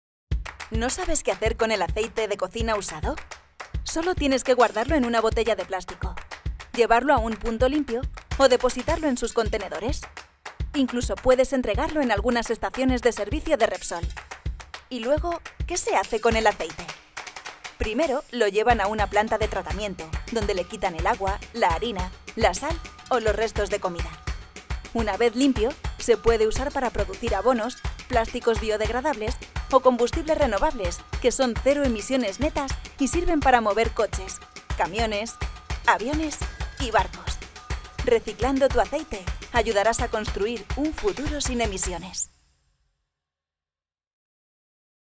Cómo se recicla el aceite de cocina usado: Pasos a seguir
El aceite es un tesoro gastronómico y una fuente de riqueza en España. Pero su valor no desaparece una vez usado en las cocinas porque aún se puede utilizar para fabricar múltiples productos, como los combustibles renovables. En este video podrás conocer cómo llega el aceite de cocina usado desde la sartén hasta el motor de tu coche.

Para reciclar el aceite usado sigue los siguientes pasos:
1. Deposita el aceite usado en botellas de plástico vacías u otros recipientes específicos, cuando esté frío.
2. Traslada el aceite a puntos de recogida, ya sean puntos limpios u ONGs especializadas. Si resides en la Comunidad de Madrid, podrás llevarlo a algunas estaciones de servicio.

Para más información consulta nuestro post que habla sobre cómo reciclar el aceite más en detalle: